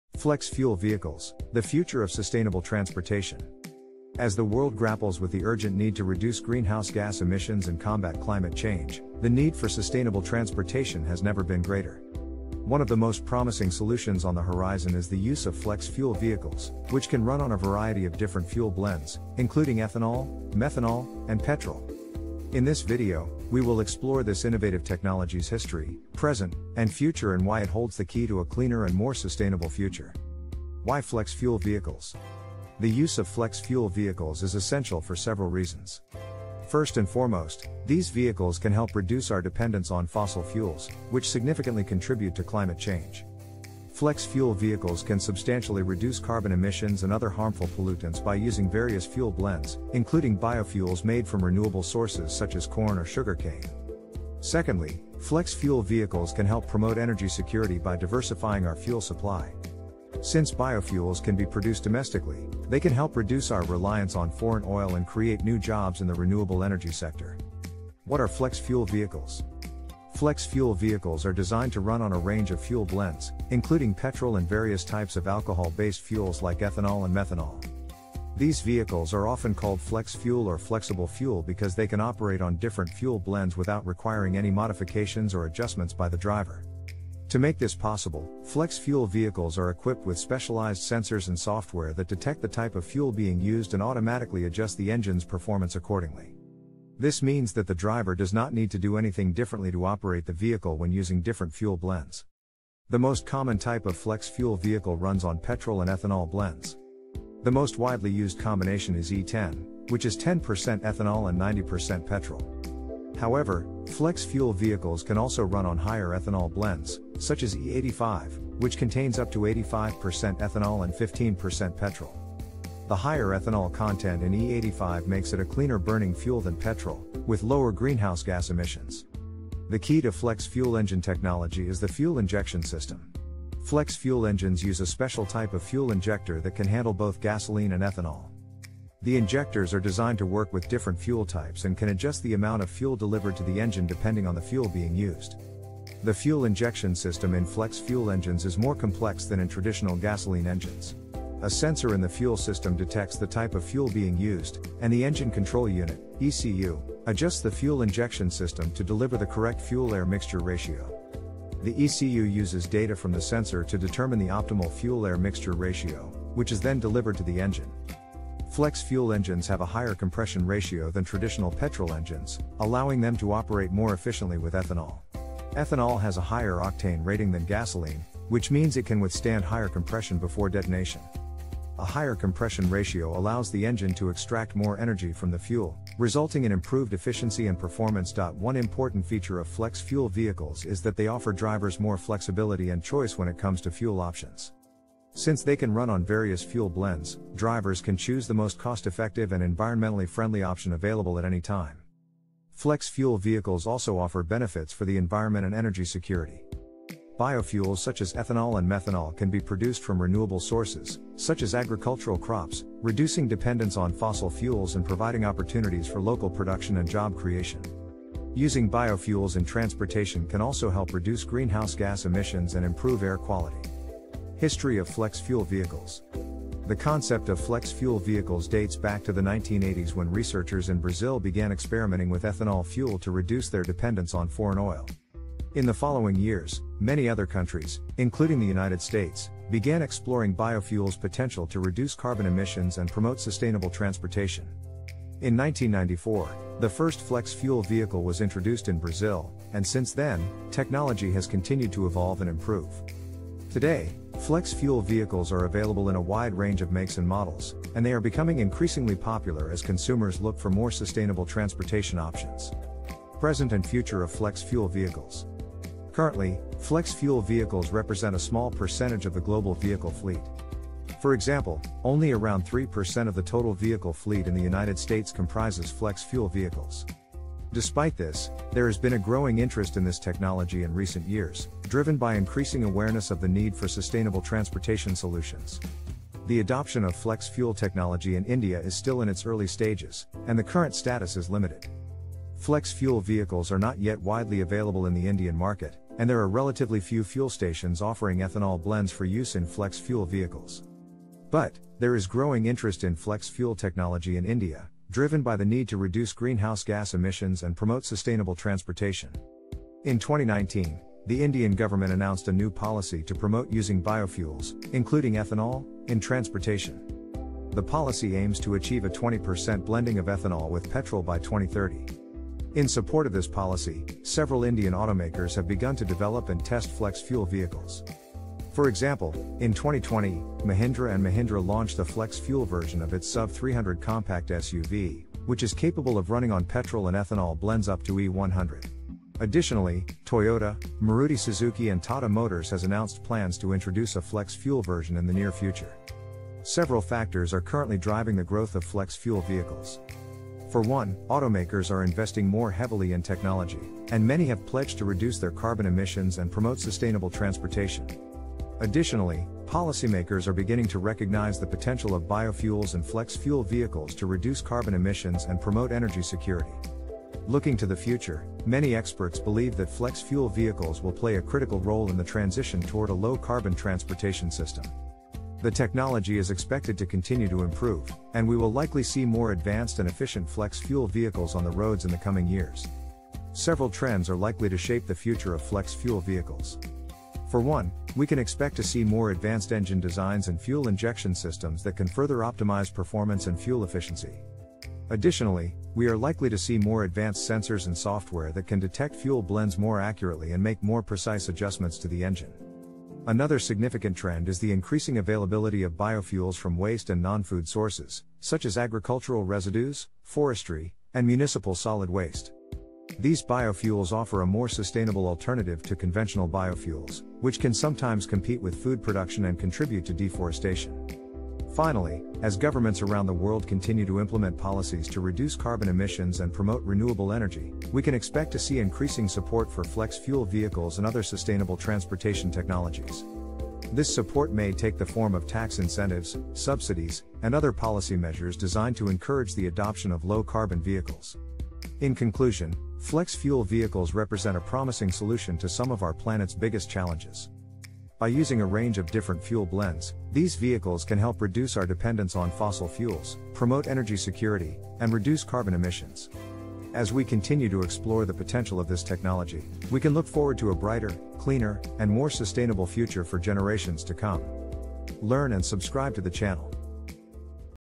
Flex Fuel Vehicles: The Future of Sustainable Transportation
In this video, we explore the technology behind flex-fuel vehicles and how they are paving the way for a sustainable future in transportation. Flex-fuel vehicles are designed to operate on a blend of petrol and ethanol, or either of them separately, making them a versatile option for drivers. We dive into the fuel injection system and how it adjusts the fuel-air mixture ratio based on the type of fuel being used, resulting in improved fuel efficiency and performance. Ethanol is a renewable fuel source made from crops such as corn, sugarcane, and wheat, which can be grown domestically, reducing our dependence on foreign oil. Additionally, the use of ethanol can reduce greenhouse gas emissions and improve air quality. Join us as we discuss the future of transportation and the role that flex-fuel vehicles will play in achieving a sustainable future.

Science made easy by simple description for all age learners.